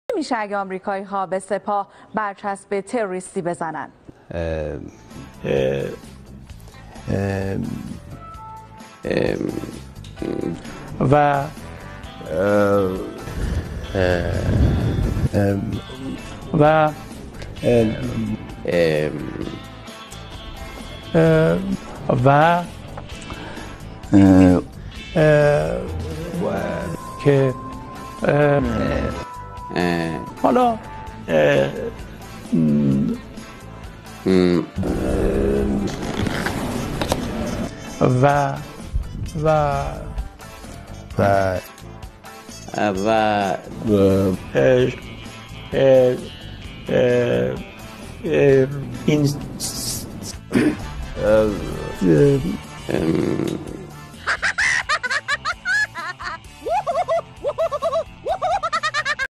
رد فعل قادة قوات الحرس الإيراني بعد تصنيفهم في لائحة المنظمات الإرهابية!!
ماذا يحصل لو صنف الأمريكان  الحرس في لائحة الإرهاب ؟!
ما بكم اصبحتم خرسان ؟؟؟